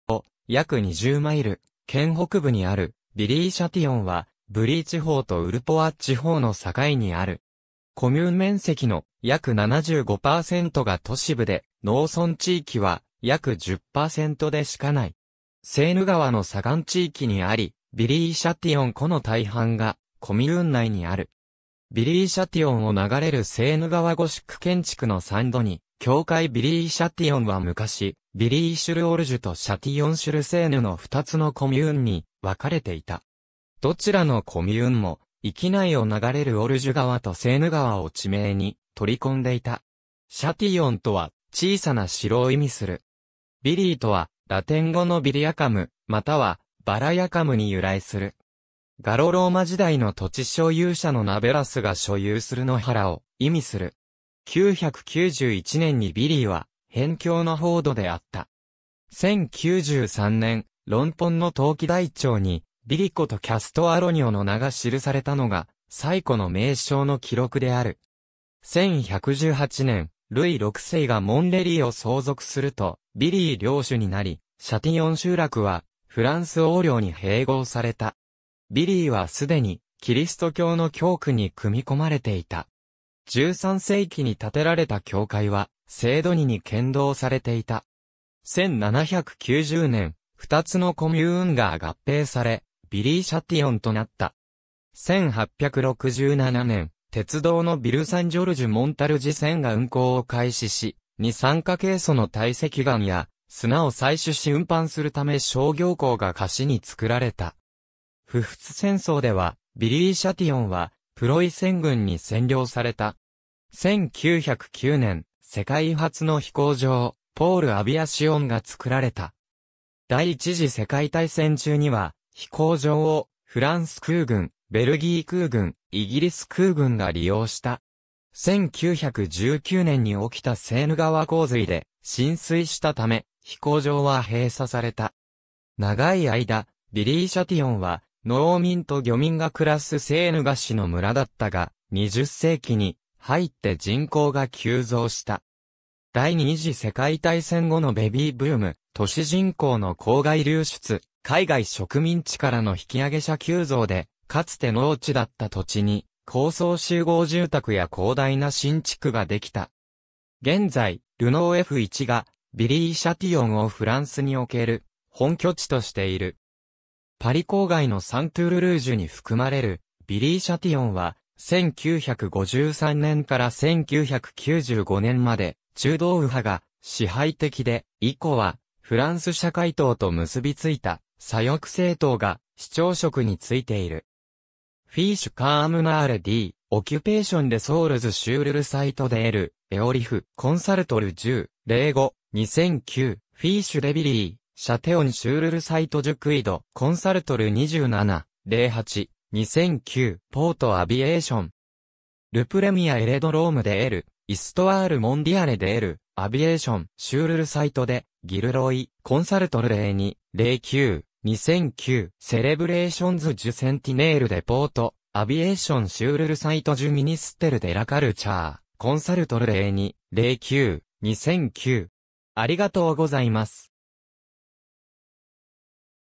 ヴィリー＝シャティヨン
ヴィリー＝シャティヨン （Viry-Châtillon）は、フランス、イル＝ド＝フランス地域圏、エソンヌ県のコミューン。
パリのノートルダム大聖堂の南東約20マイル、県北部にあるヴィリー＝シャティヨンは、ブリー地方とウルポワ地方の境にある。
コミューン面積の約75%が都市部で、農村地域は約10%でしかない。
セーヌ川の左岸地域にあり、ヴィリー＝シャティヨン湖の大半がコミューン内にある（残りはグリニーに属す）。
ヴィリー＝シャティヨンを流れるセーヌ川 ゴシック建築のサン＝ドニ教会 ヴィリー＝シャティヨンは昔、ヴィリー＝シュル＝オルジュ（Viry-sur-Orge）とシャティヨン＝シュル＝セーヌ（Châtillon-sur-Seine）の2つのコミューンに分かれていた。
どちらのコミューンも、域内を流れるオルジュ川とセーヌ川を地名に取り込んでいた。
シャティヨンとは『小さな城』を意味する。
ヴィリーとはラテン語のViriacumまたはVeriacumに由来する。
ガロ＝ローマ時代の土地所有者の名Verusが所有する野原を意味する。
991年にヴィリーは辺境の封土であった。
1093年、ロンポンの登記台帳にViriacoとCastellonioの名が記されたのが最古の名称の記録である。
1118年、ルイ6世がモンレリーを相続すると、ヴィリー領主になり、シャティヨン集落はフランス王領に併合された。
ヴィリーは既にキリスト教の教区に組み込まれていた。
13世紀に建てられた教会は聖ドニに献堂されていた。
1790年、2つのコミューンが合併されヴィリー＝シャティヨンとなった。
1867年、鉄道のヴィルヌーヴ＝サン＝ジョルジュ-モンタルジ線が運行を開始し、二酸化珪素の堆積岩や、砂を採取し運搬するため商業港が河岸につくられた。
普仏戦争ではヴィリー＝シャティヨンはプロイセン軍に占領された。
1909年、世界初の飛行場ポール・アヴィアシオン（fr）がつくられた · 。
第一次世界大戦中には、飛行場をフランス空軍、ベルギー空軍、イギリス空軍が利用した。
1919年に起きたセーヌ川洪水で浸水したため、飛行場は閉鎖された。
長い間、ヴィリー＝シャティヨンは農民と漁民が暮らすセーヌ河岸の村だったが、20世紀に入って人口が急増した。
第二次世界大戦後のベビーブーム、都市人口の郊外流出、海外植民地からの引揚者急増（ピエ・ノワールなど）で、かつて農地だった土地に高層集合住宅（fr）や広大な新地区ができた。
現在、ルノーF1がヴィリー＝シャティヨンをフランスにおける本拠地としている。
パリ郊外のサントゥール・ルージュ（fr、共産主義者市長が多い郊外コミューンの総称）に含まれるヴィリー＝シャティヨンは、1953年から1995年まで中道右派が支配的で、以降はフランス社会党と結びついた左翼政党が市長職についている。
^ Fiche communale d’occupation des sols sur le site de l’Iaurif. Consulté le 10/05/2009. ^ « Fiche de Viry-Châtillon sur le site du Quid. » (Archive • Wikiwix • Archive.is • Google • Que faire ?) Consulté le 27/08/2009. ^ Port-Aviation,
le premier aérodrome de l’Histoire mondiale de l’aviation. sur le site de Gil Roy. Consulté le 02/09/2009. ^ Célébrations du centenaire de Port-Aviation sur le site du ministère de la Culture. Consulté le 02/09/2009.